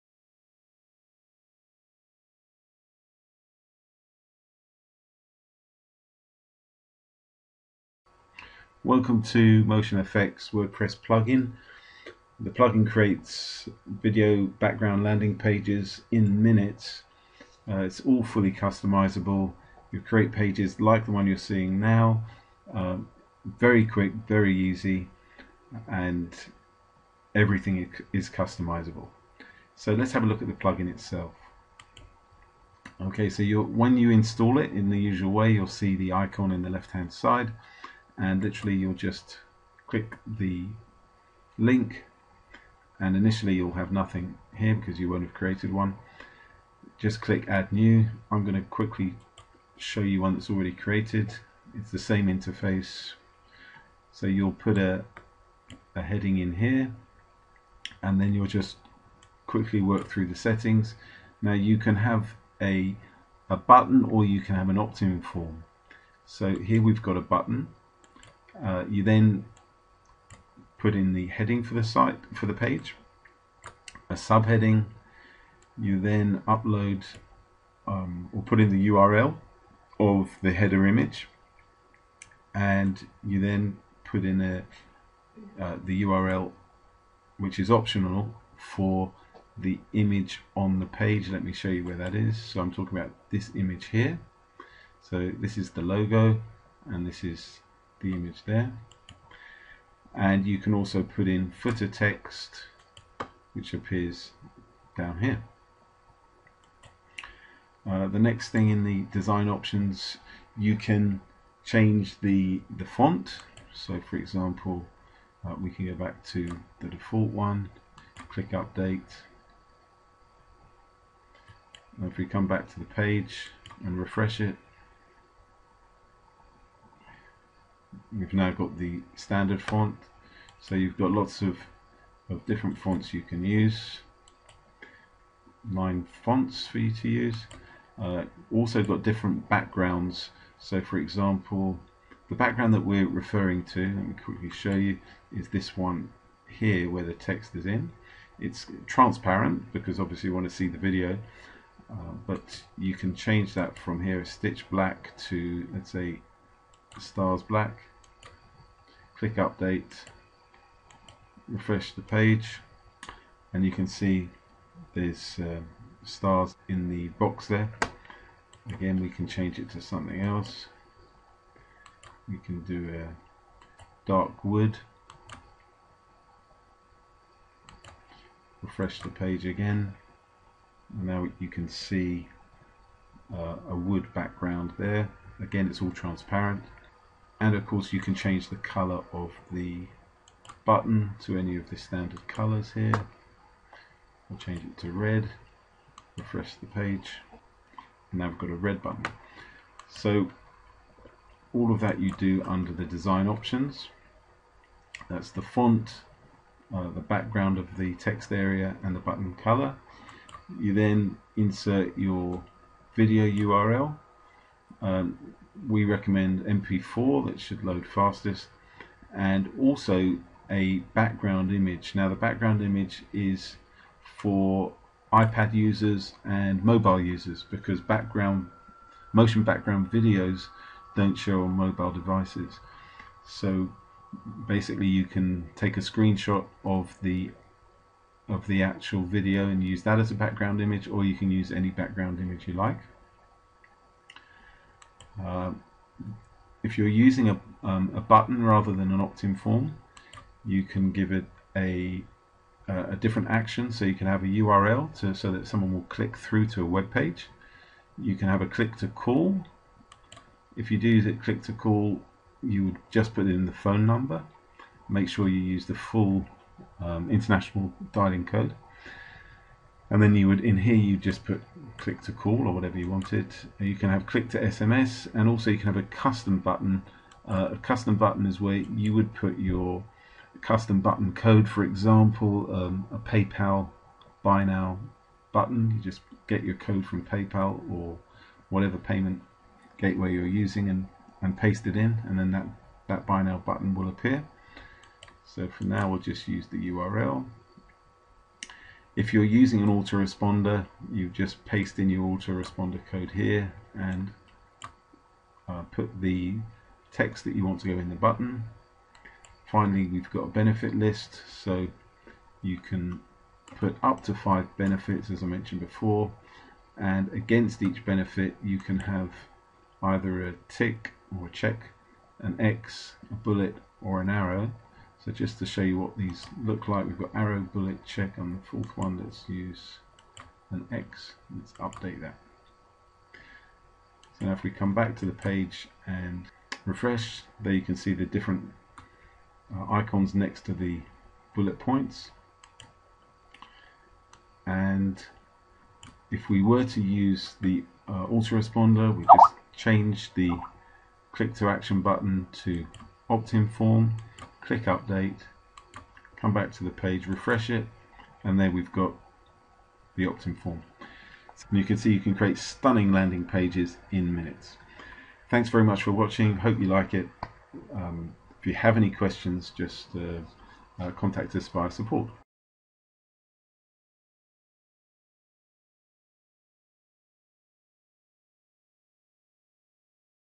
MotionFX Wordpress Plugin
How to create video motion background landing pages with MotionFX Wordpress Plugin.